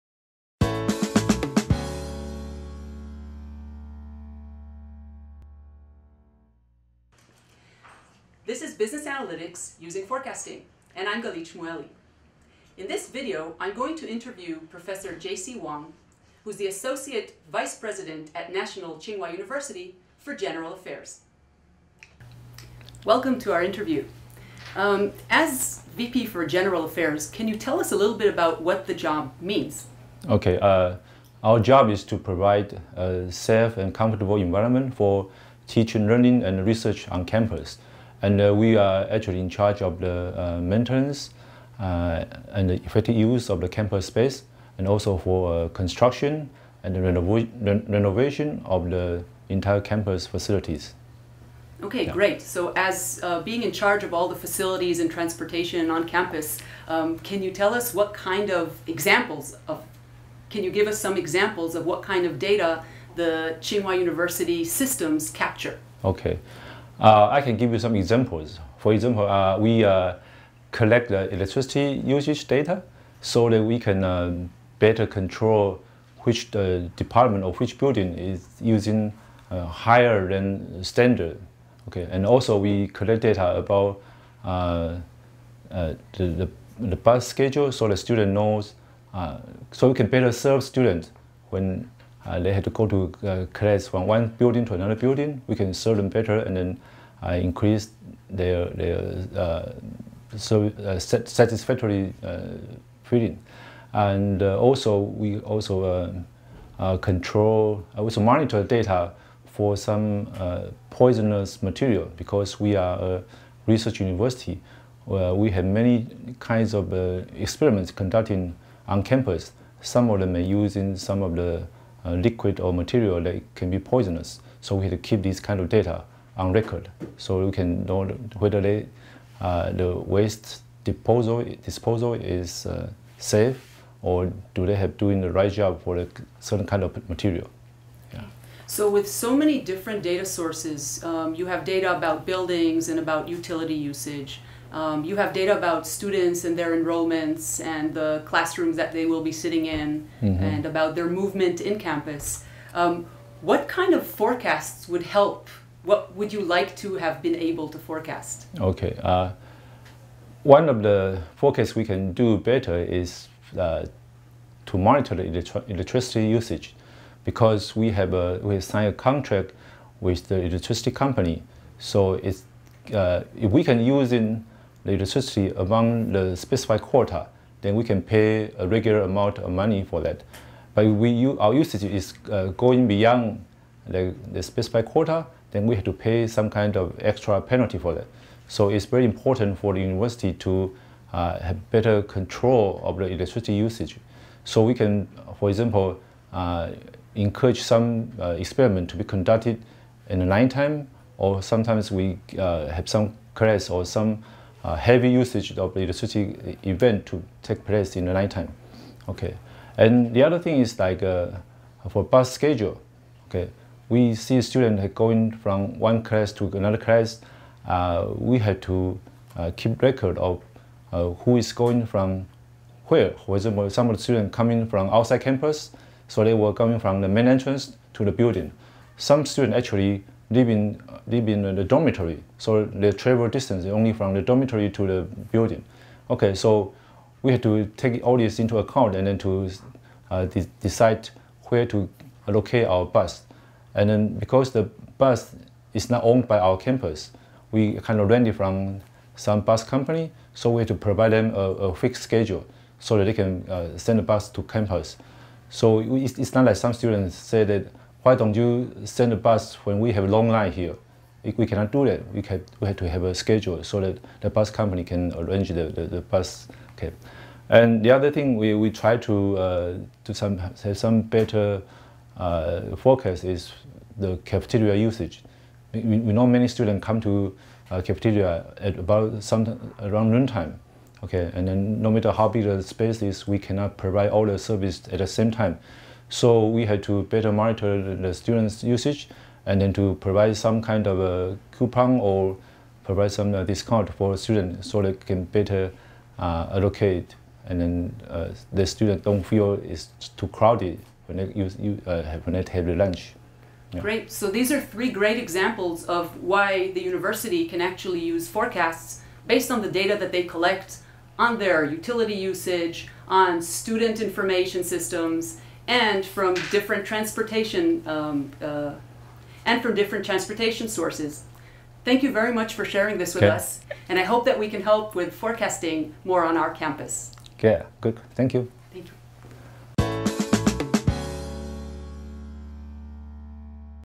Interviews: Interview with NTHU VP for General Affairs about forecasting
Interview with Prof. JC Wang, VP for General Affairs at NTHU about what data is collected at NTHU and potentially useful forecasting solutions

This video supports the textbook Practical Time Series Forecasting.